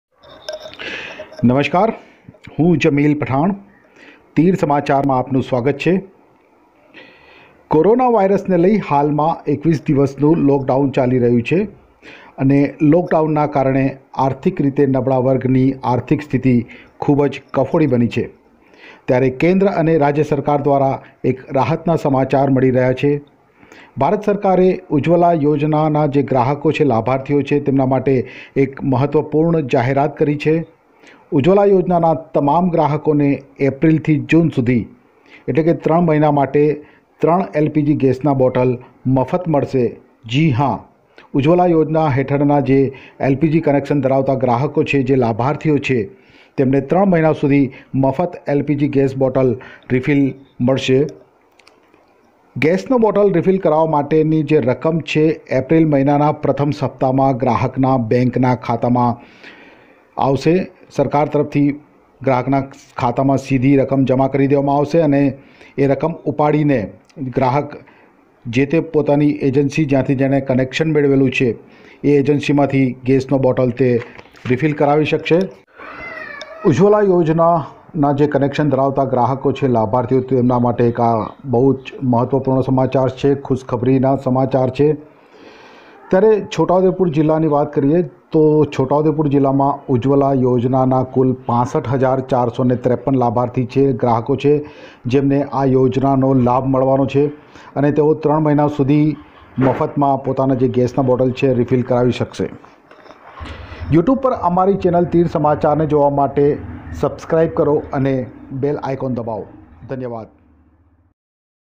Ujjavala Yojna Ke Sabhi Grahako Ke Liye Badi Khabar..
Ujjavala yojnana Grahako ne tran mahina sudhi malshe free gas bottle.